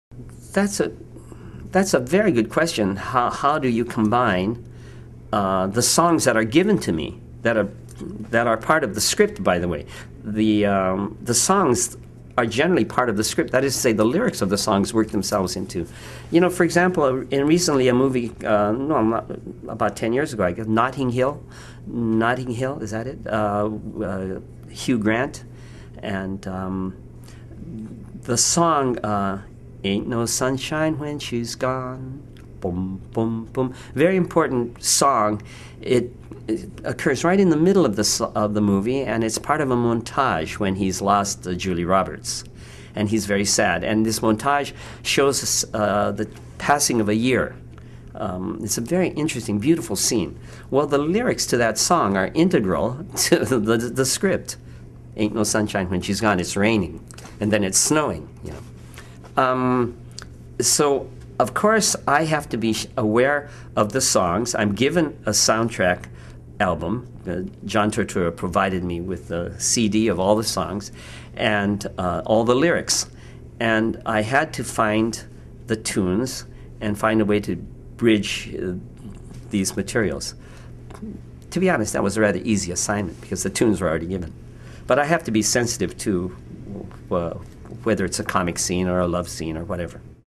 Paul Chihara On Integrating Existing Songs into a Films Score
- Paul Chihara, Professor of Composition at UCLA, points out some of the things a film composer/scorer must keep in mind when integrating an existing song into the film.